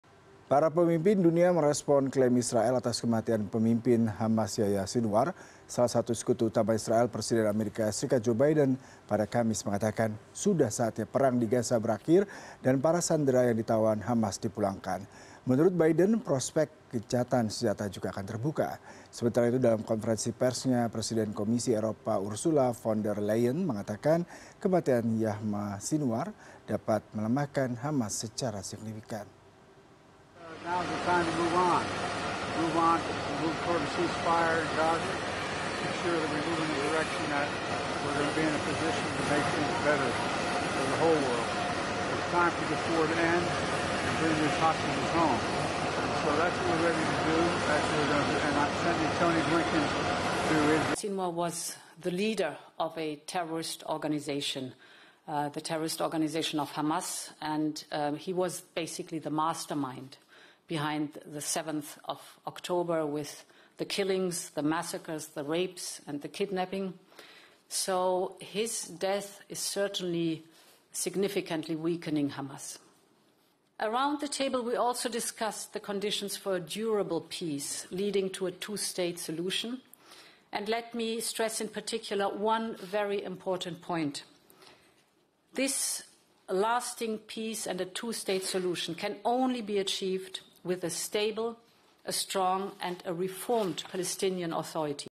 Para Pemimpin Dunia Tanggapi Klaim Kematian Pemimpin Hamas Yahya Sinwar
Para pemimpin dunia merespons klaim Israel atas kematian pemimpin Hamas, Yahya Sinwar. Salah satu sekutu utama Israel, Presiden Amerika Serikat Joe Biden, pada Kamis menyatakan sudah saatnya perang di Gaza berakhir dan para sandera yang ditawan Hamas dipulangkan. Menurut Biden, prospek gencatan senjata juga akan terbuka. Sementara itu, dalam konferensi persnya, Presiden Komisi Eropa Ursula von der Leyen mengatakan kematian Yahya Sinwar dapat melemahkan Hamas secara signifikan.